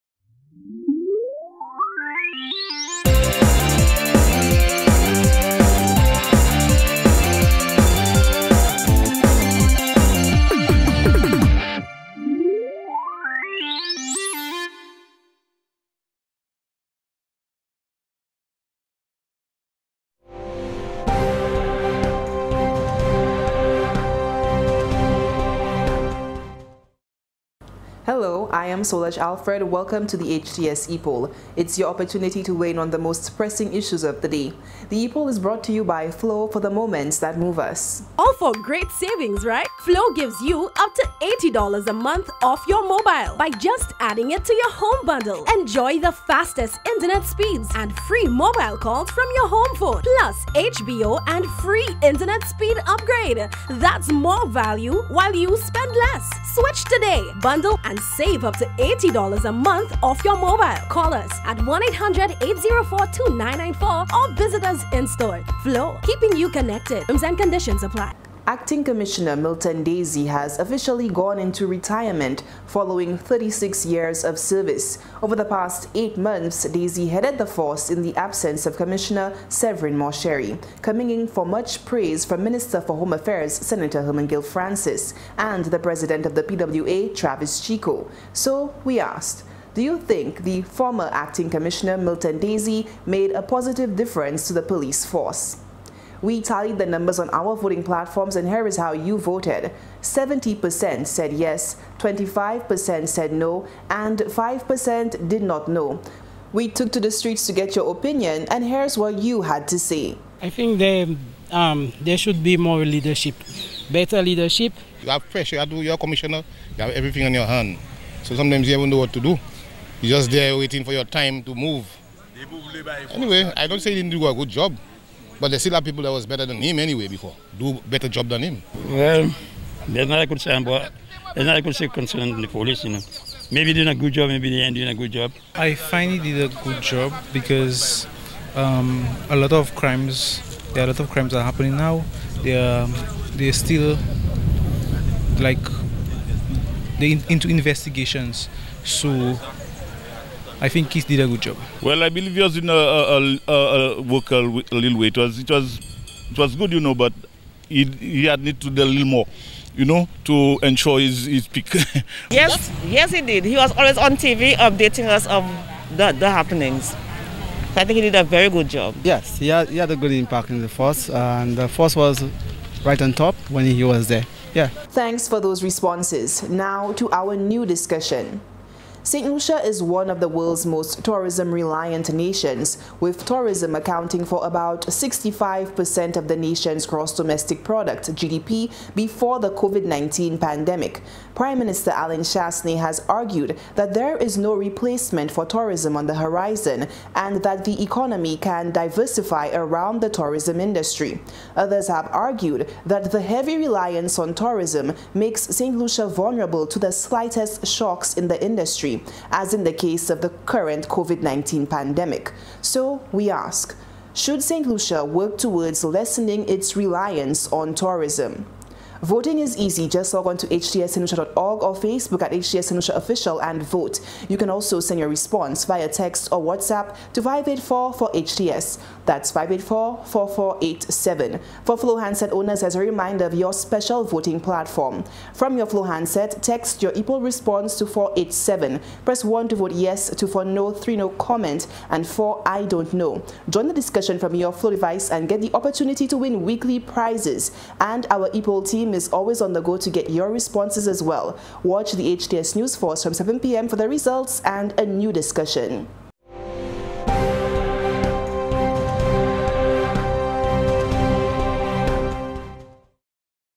HTS EPOLL SEPTEMBER 14TH 2020
Should St. Lucia work towards lessening its reliance on tourism?